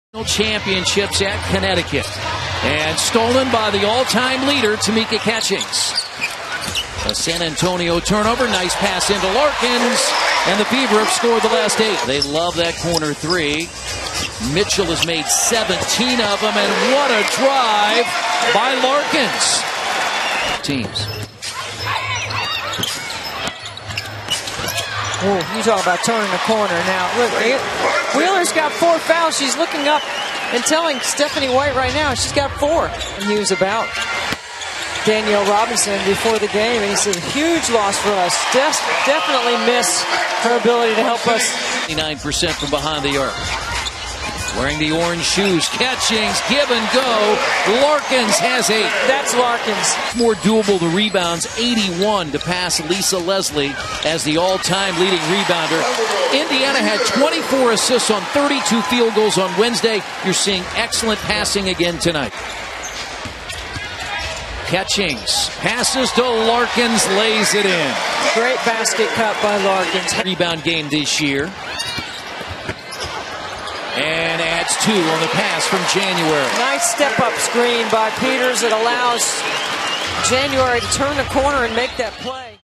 Postgame: Erlana Larkins Highlights - 160701
Erlana Larkins posts 16 points and 17 rebounds in the Fever's 87-85 overtime loss to the Stars.